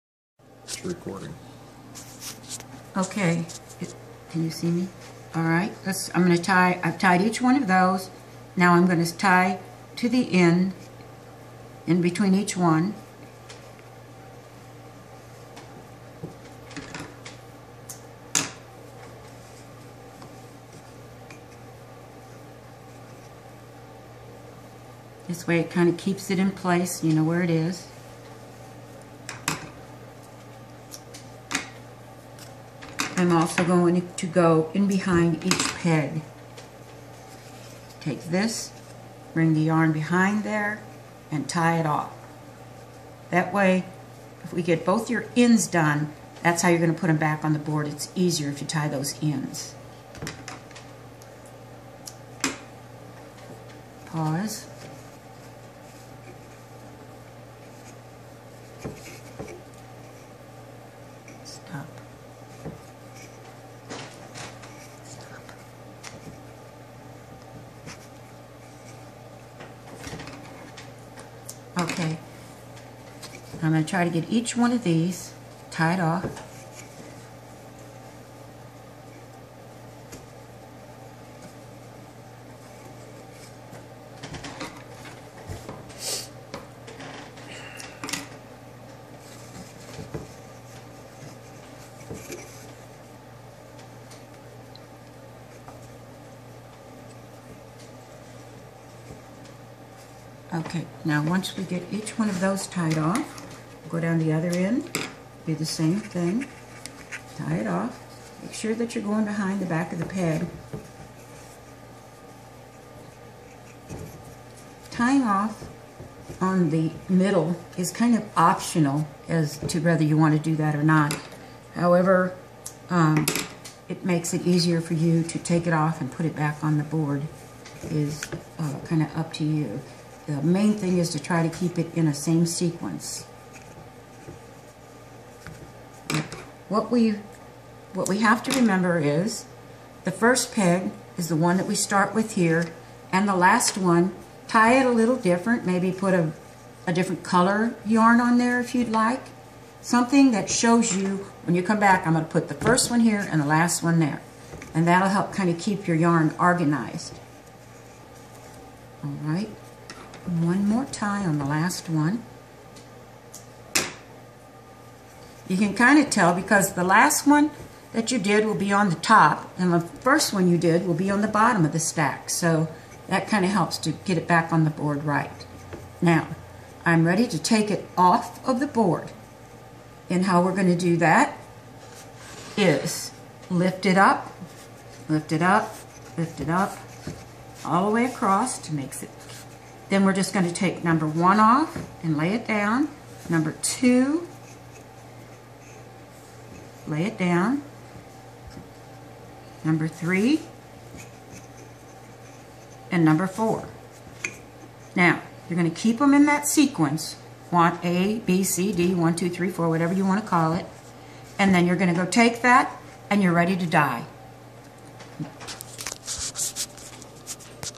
Dye Board Yarn Farm tying warp off video 2 of one
Dying off on the dye board is important to keep the yarn organized in stripes to dye.